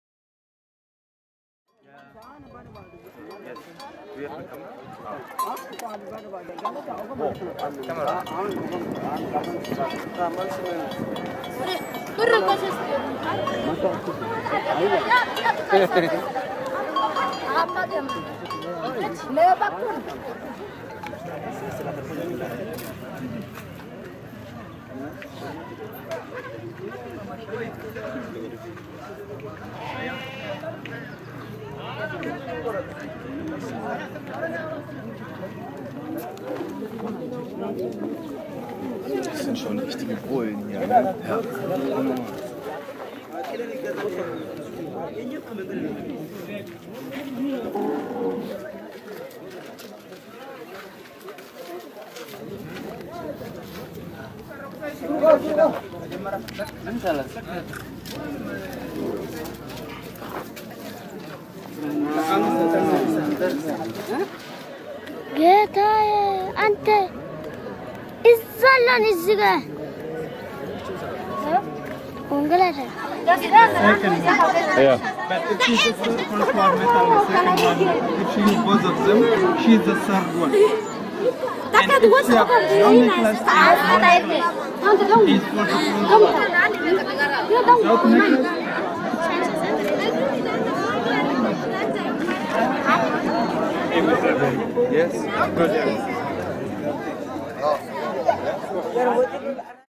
Key Afer / Äthiopien / auf dem Markt 2015
Key Afer liegt in den Banna-Bergen im Süden Äthiopiens. Den Namen hat der Ort nach dem roten Boden, der überall da aufleuchtet, wo kein Bewuchs ihn bedeckt. Auf dem Markt in Key Afer treiben gleich vier Stämme regen Handel untereinander. Die Bena, Ari, Hamer und die Tsemay.